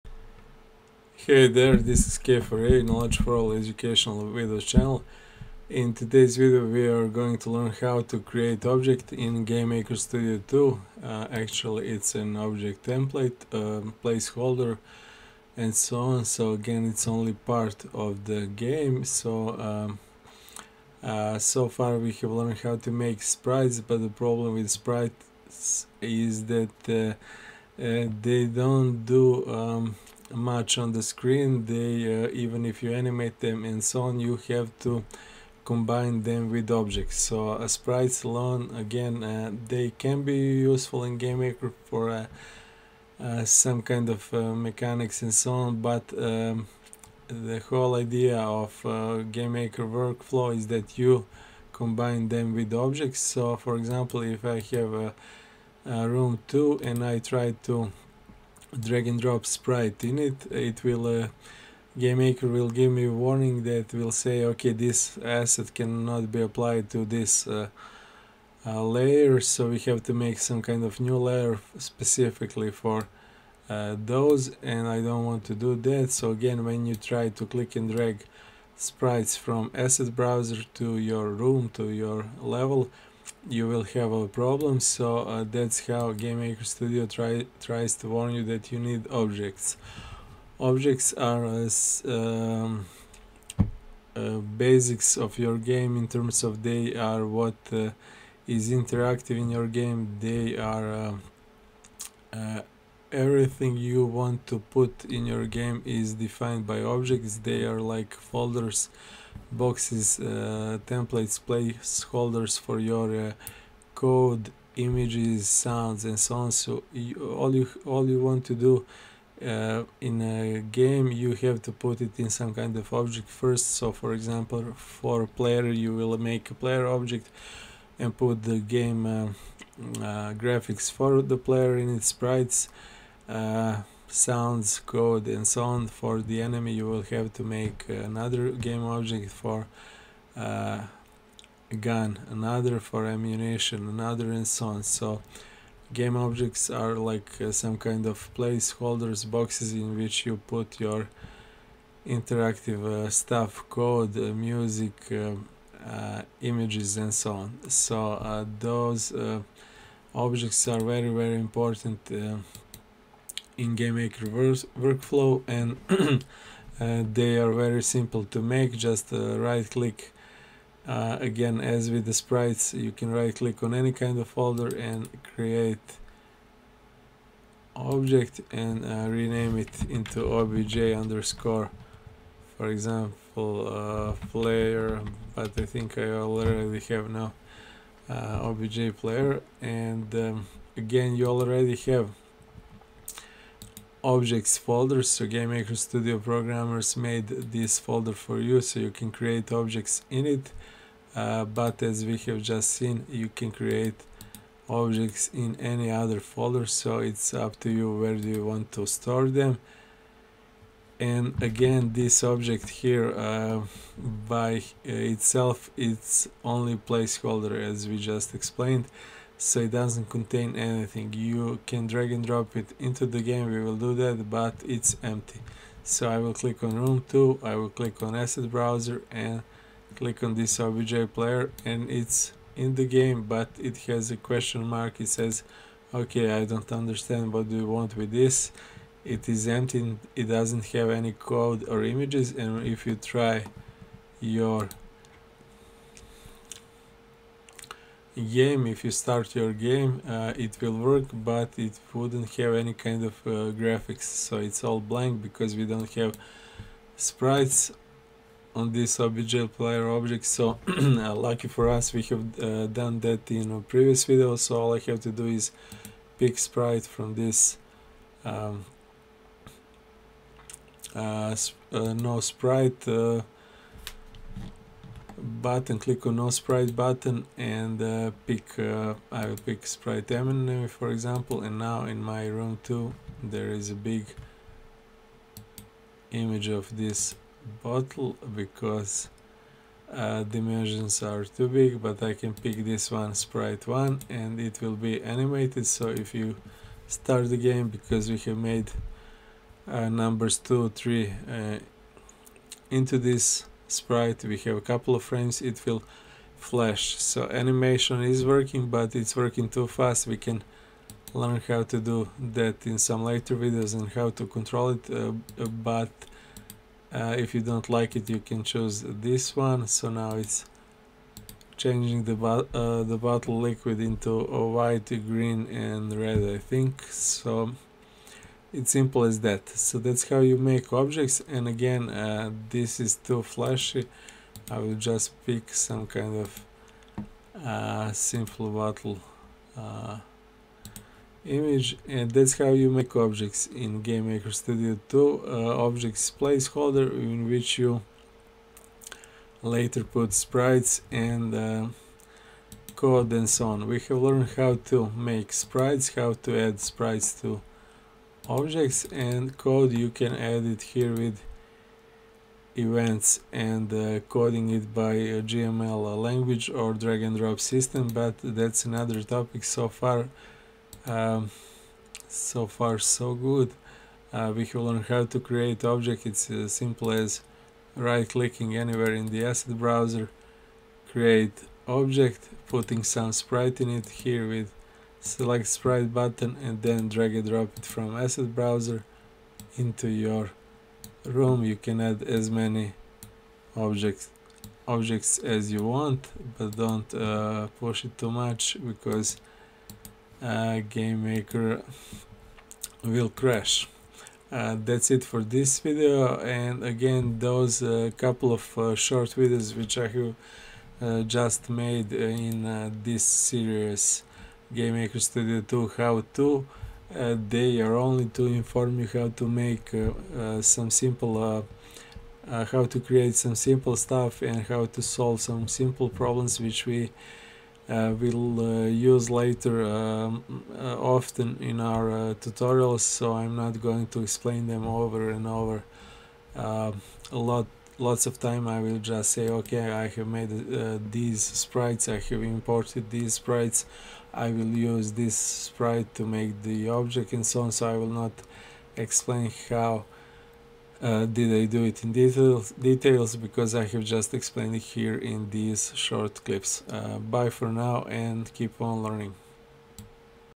GameMaker - How To Create Object
Creating Sprite and importing image for it is only first step in creation.
After that you need to create Object as placeholder for that Sprite, code, sound,..., which will be attached to it if needed.

You will do this a lot in your GameMaker development, and all you have to do is:
- right click somewhere in Asset Browser
- Create - Object

And that's it, you have created empty Object !
Now name it, select some Sprite for it and drag it into your game Room to make it visible in your game !